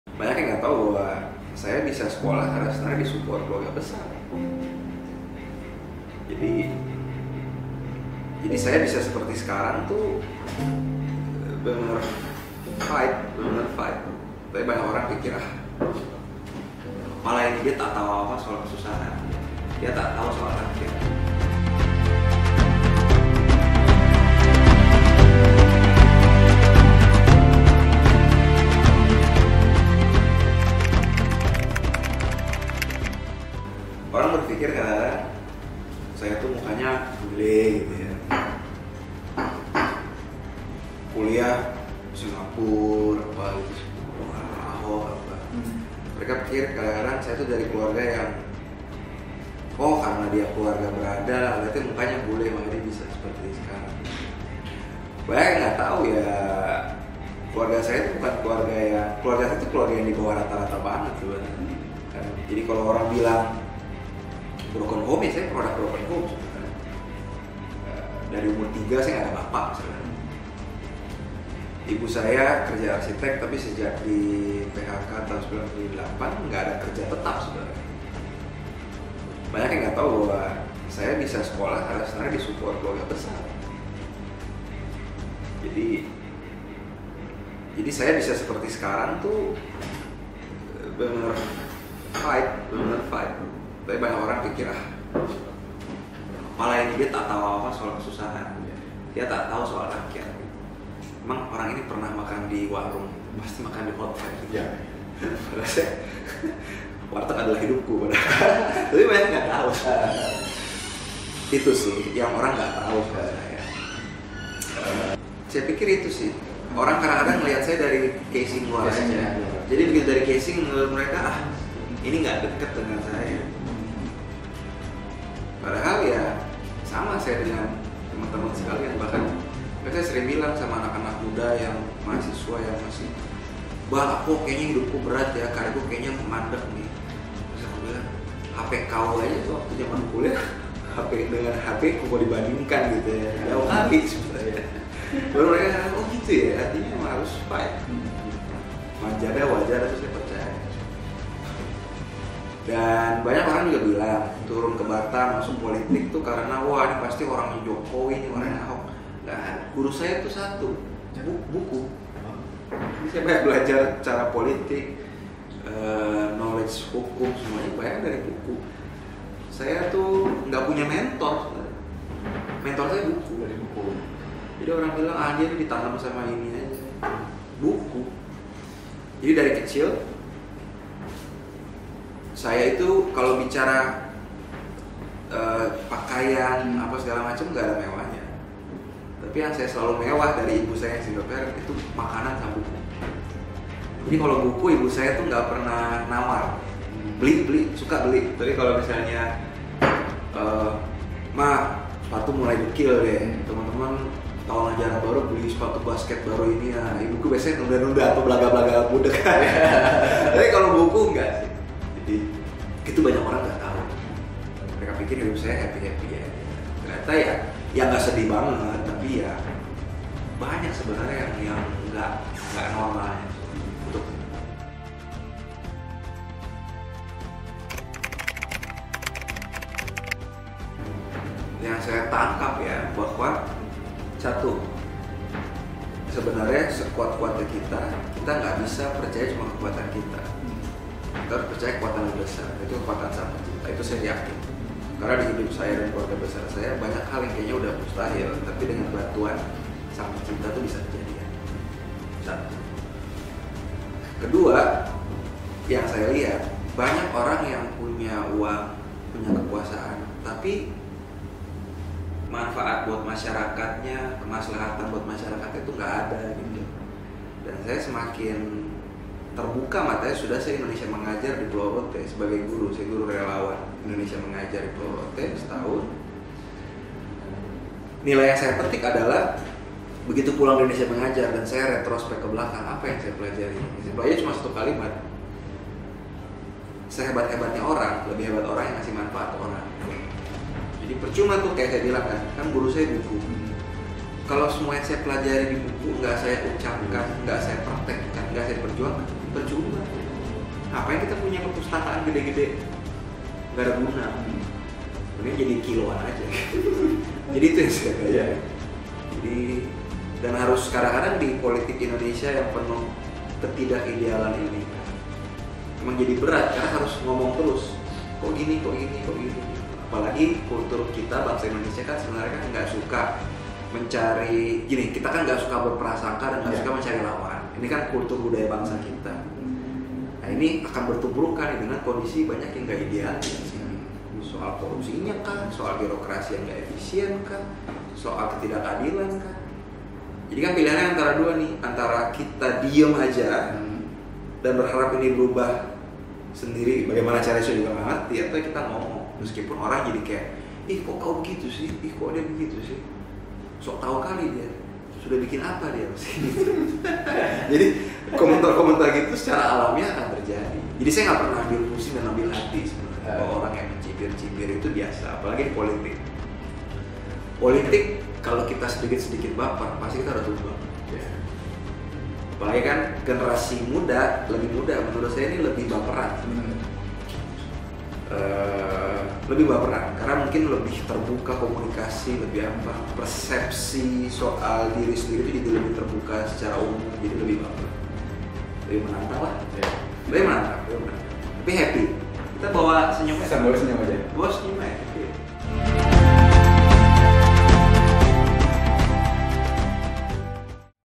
Q&A Rian Ernest PART 1: Bukan Jokowi dan Ahok, Guru Saya Buku
TRIBUNBATAM.id - Rian Ernest yang merupakan mantan juru bicara Tim Kampanye Nasional (TKN) Jokowi-Ma'ruf telah mendeklarasikan diri maju independen di pilwako Batam 2020.

Keseriusan tersebut juga telah ditunjukkan oleh Rian Ernest dengan membentuk gerakan Batam Baru dan relawan Batam Baru untuk mengumpulkan dukungan.

Politisi Partai Solidaritas Indonesia (PSI) ini, sudah menggandeng salah satu tokoh Batam, Yusiani Gurusinga sebagai wakilnya.

Keduanya sudah bergerilya turun langsung menghimpun dukungan untuk maju lewat jalur perseorangan.

Berikut Wawancara khusus Rian Ernest di chanel youtube TRIBUNBATAM.id.